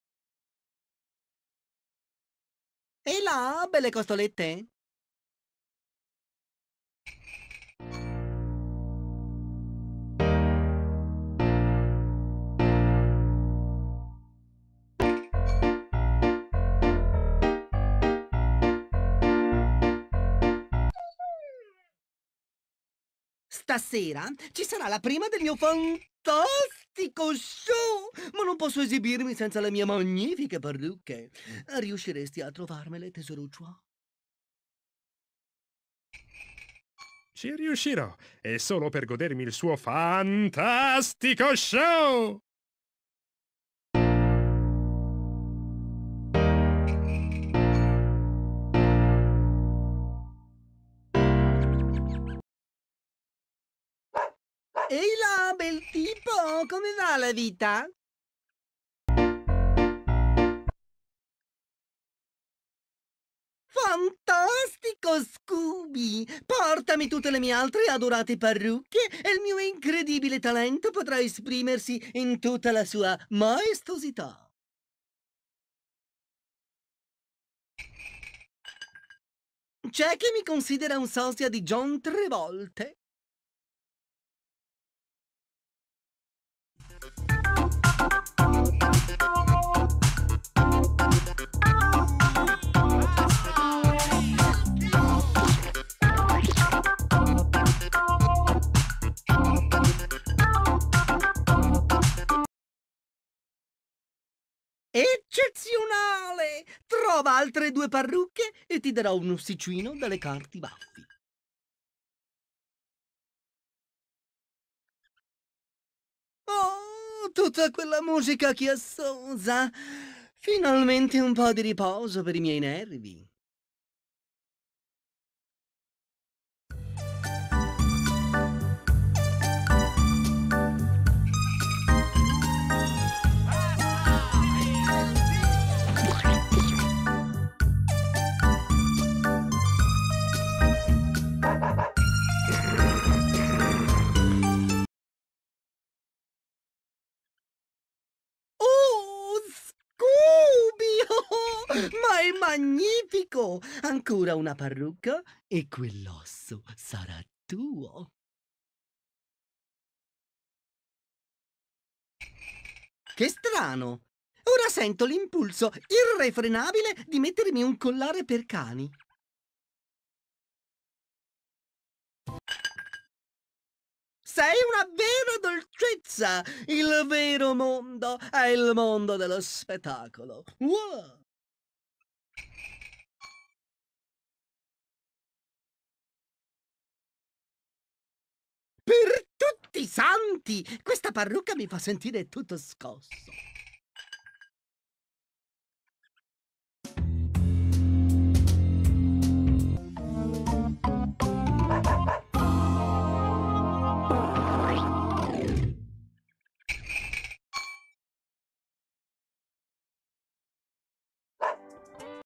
Stereotipo INVECCHIATO MALE
Prima senti la battuta del collare per cani, poi ti ricordi che il gioco è PEGI 3+.